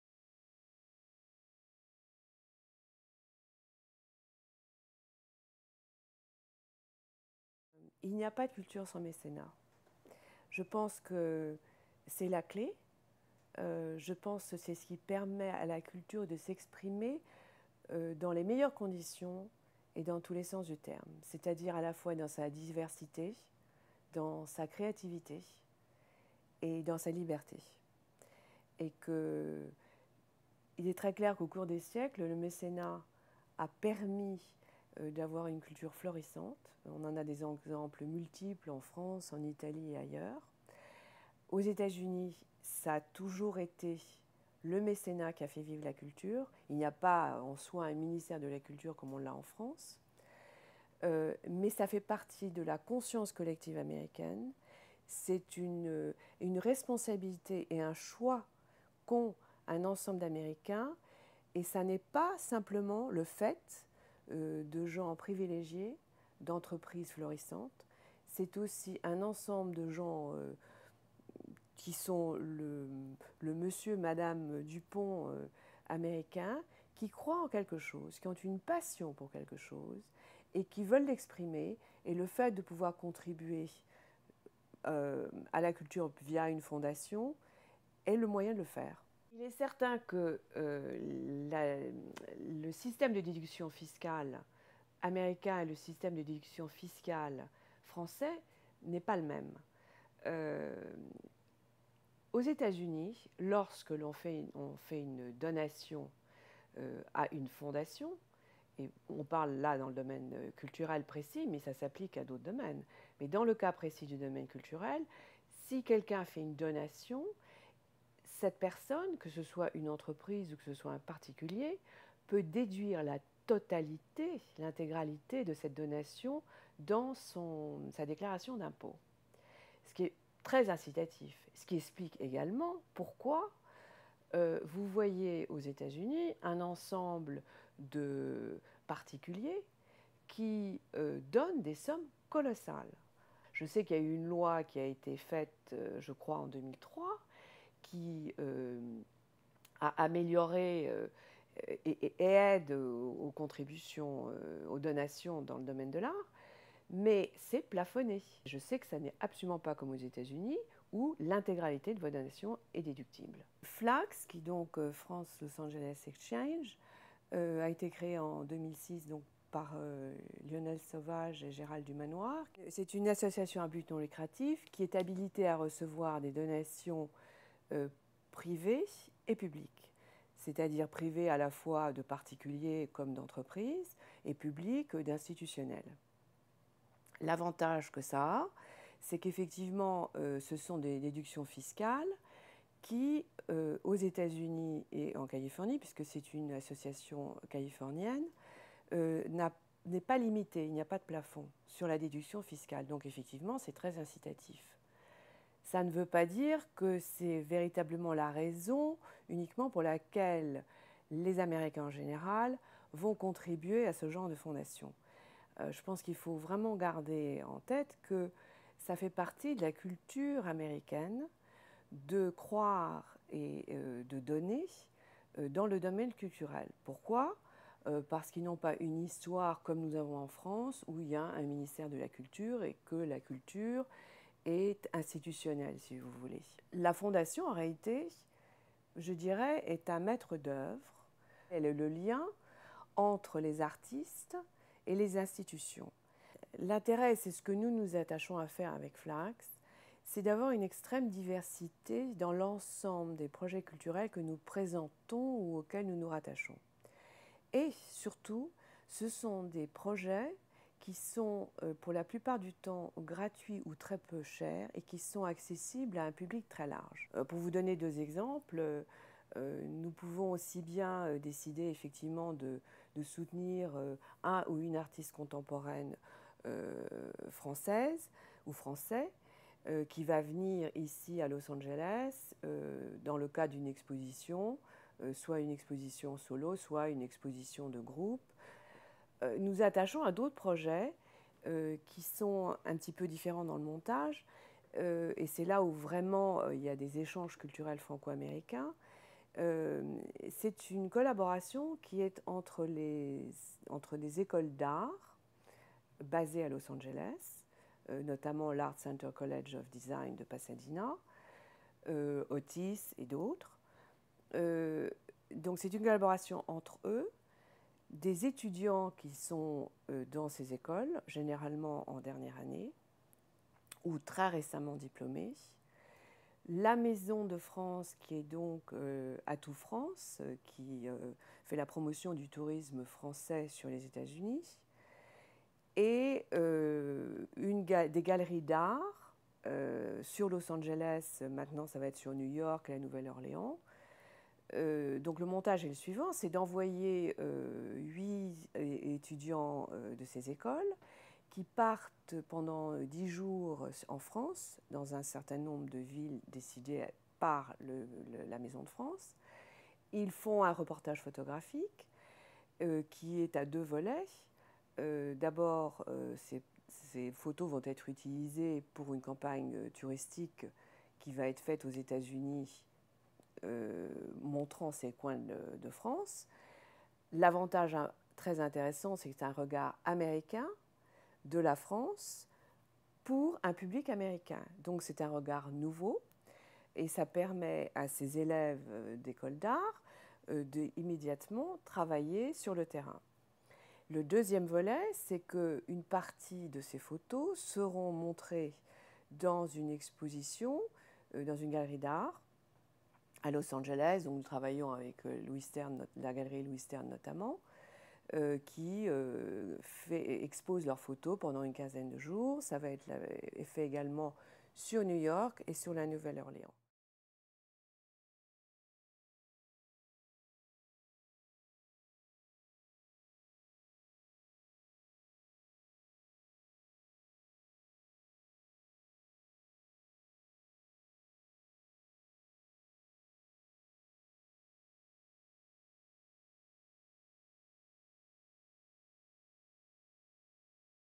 Elisabeth Forney - Pas de culture sans mécénat - Forum d'Avignon 2009
"Pas de culture sans mécènat - La fiscalité Grande Bretagne/France"

Elisabeth Forney, Directeur FLAX

Thème : Création et innovation pour un nouveau monde